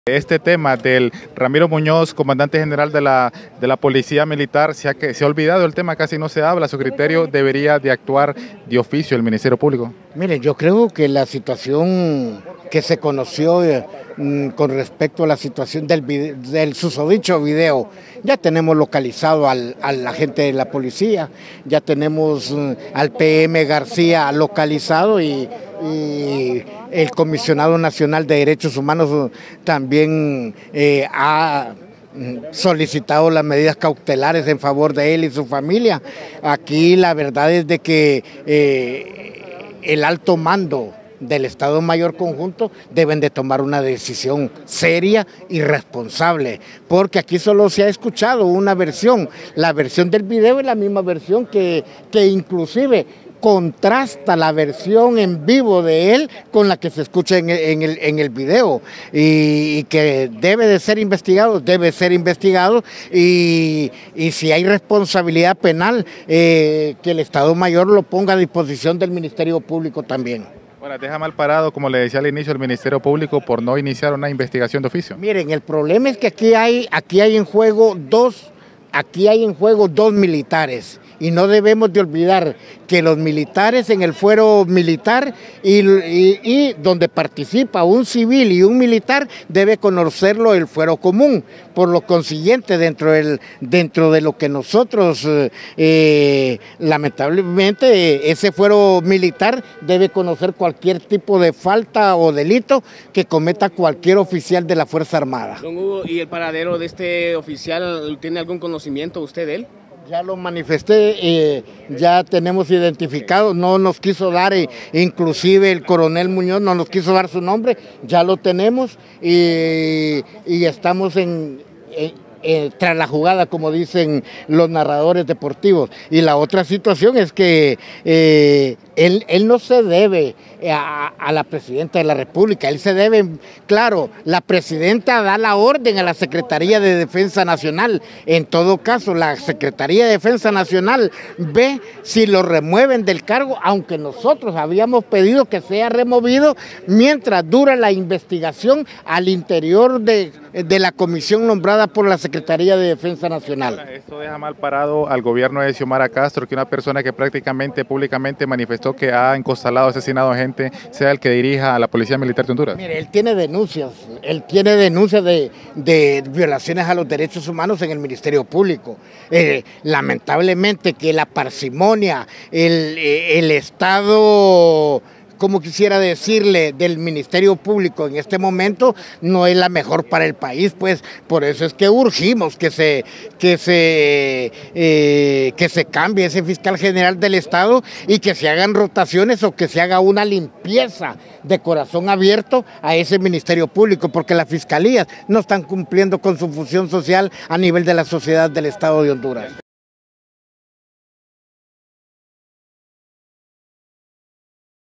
Señalan que FF.AA., debería tomar acciones por amenazas de director de la PM contra subalterno
El presidente del Comité para la Defensa de los Derechos Humanos de Honduras (Codeh), Hugo Maldonado, señaló que el alto mando del Estado Mayor Conjunto debería tomas acciones concretas por las amenazas que realizó el director de la Policía Militar (PM), Ramiro Muñoz, contra un subalterno.